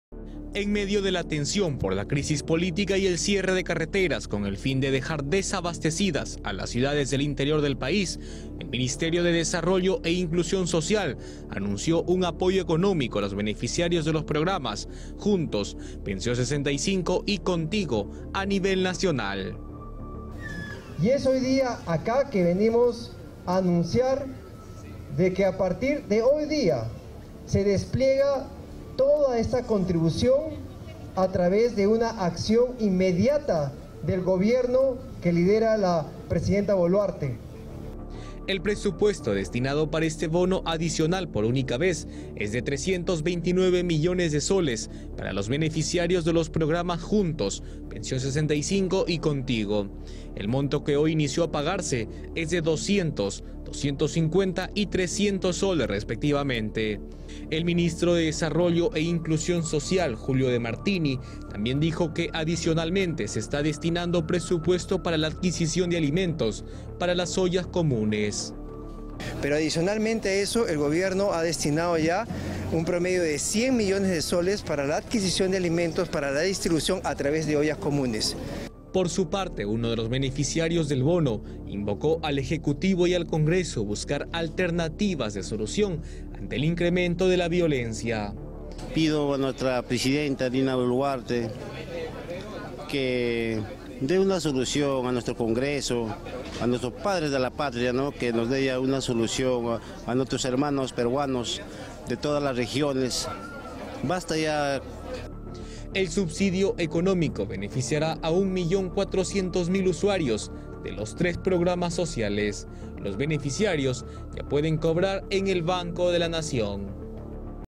Ministro Demartini anuncia inicio de apoyo económico a usuarios de Juntos, Pensión 65 y Contigo
En el marco de las violentas manifestaciones que tienen al Perú bajo ataque, el ministro de Desarrollo e Inclusión Social (Midis), Julio Demartini, anunció el inicio del pago de la subvención adicional extraordinaria que beneficiará a cerca de 1.4 millones de usuarios de los programas sociales "Juntos", "Pensión 65" y "Contigo".

Esta medida, que forma parte del plan "Con Punche Perú" del Gobierno de transición, busca reactivar la economía de todas aquellas personas en situación de vulnerabilidad.

Cabe precisar que para concretar el pago de este bono adicional, el Gobierno ha designado un presupuesto de 329 millones de soles en beneficio de los usuarios de los 3 programas sociales y ya se empezó a pagar los montos de 200, 250 y 300 soles.

Apoyo a ollas comunes

Asimismo, el ministro Julio Demartini anunció que adicionalmente a este apoyo económico, se estará destinando presupuesto para la adquisición de alimentos y víveres de primera necesidad en beneficio de las ollas comunes.